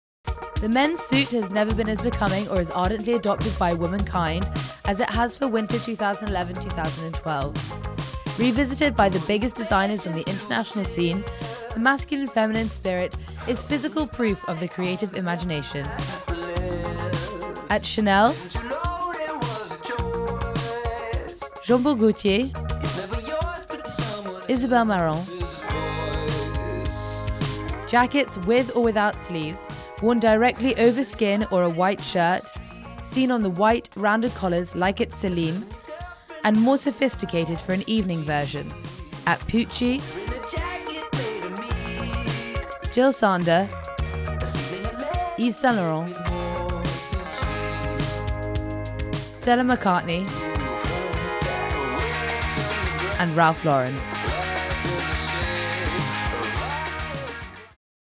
Trend: Androgyny - Fall/Winter 2011/12
The men's suit has been adopted by all women this winter season 2011/12.